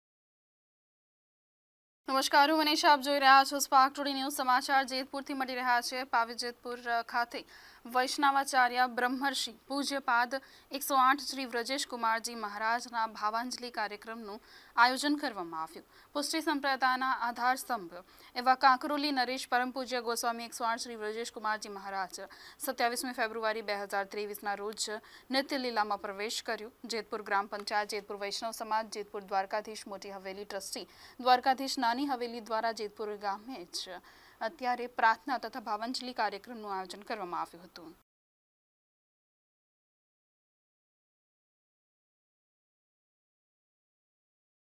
વૈષ્ણવાચાર્ય બ્રહ્મર્ષિ પૂજ્ય પાદ ૧૦૮ શ્રી વ્રજેશકુમારજી મહારાજશ્રીનો ભાંવાજલી કાર્યક્ર્મ 2023
જેતપુર પાવી ખાતે વૈષ્ણવાચાર્ય બ્રહ્મર્ષિ પૂજ્ય પાદ ૧૦૮ શ્રી વ્રજેશકુમારજી મહારાજશ્રીનો ભાંવાજલી કાર્યક્ર્મ